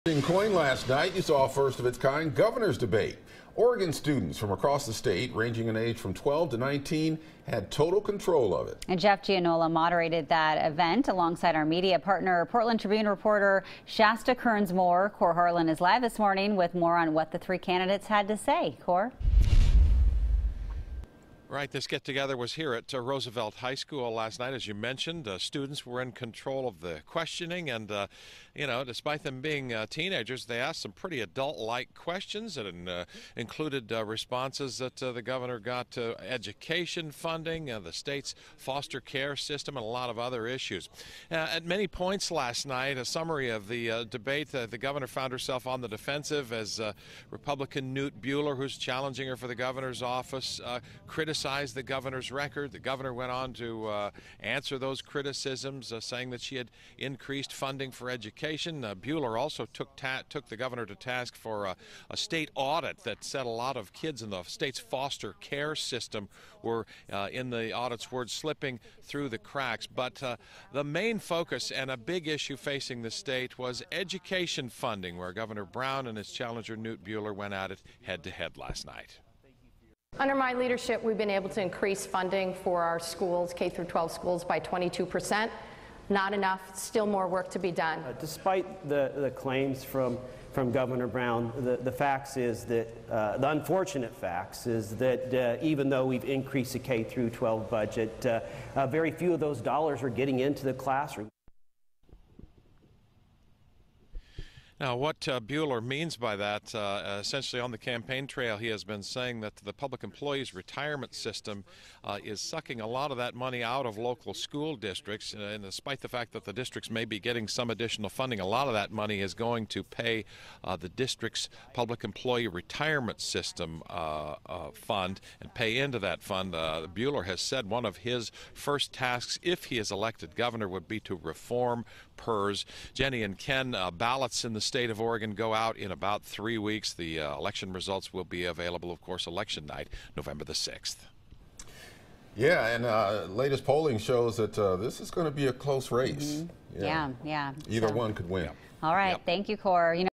Governor candidates 'Debate for Oregon's Future'
The Debate for Oregon’s Future, the first of its kind of debate, happened Tuesday. All three Governor candidates -- Kate Brown, Knute Buehler and Patrick Starnes -- answered questions from students across Oregon regarding a variety of different topics.